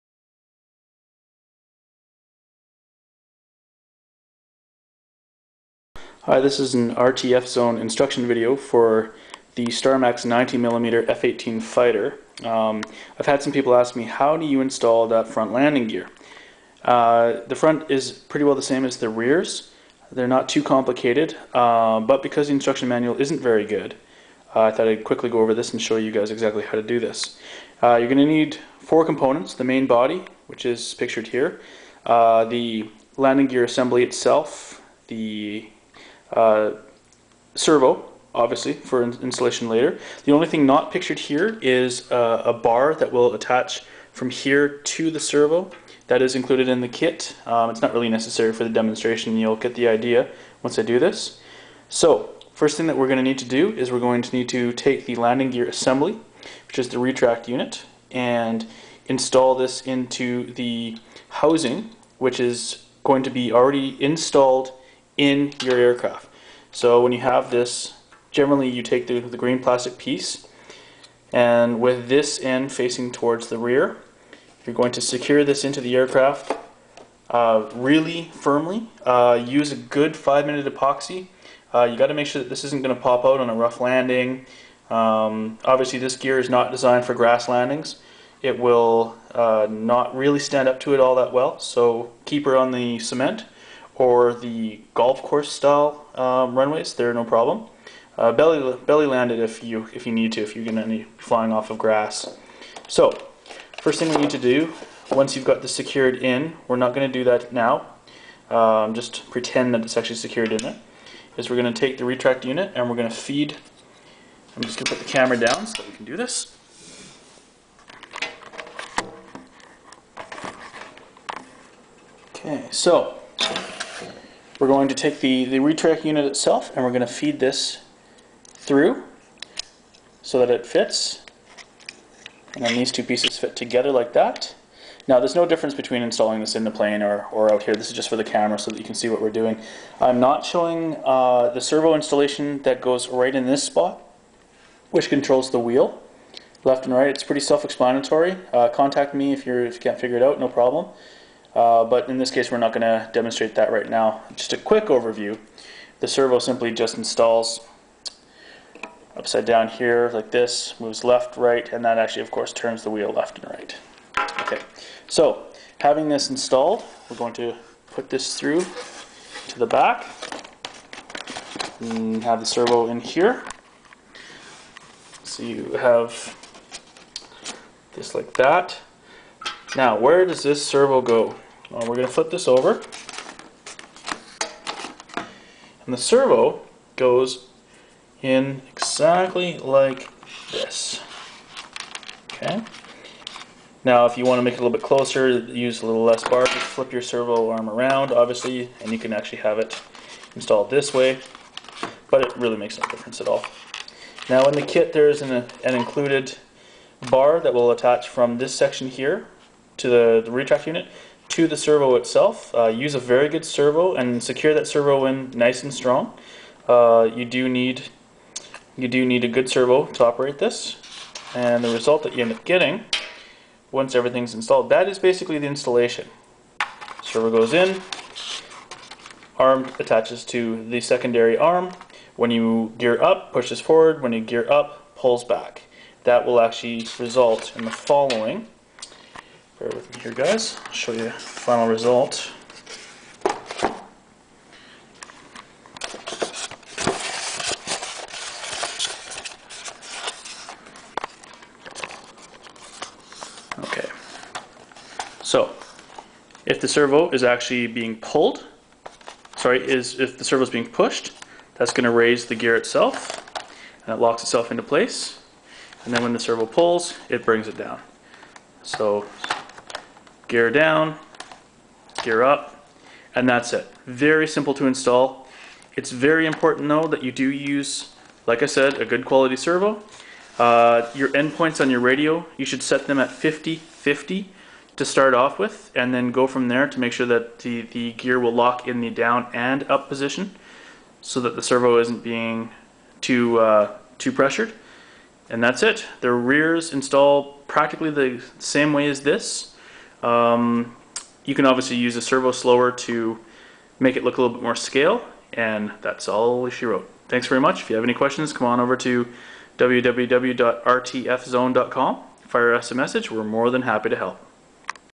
F-18 90mm retracts
Starmax F-18 90mm Landing Gear Installation overview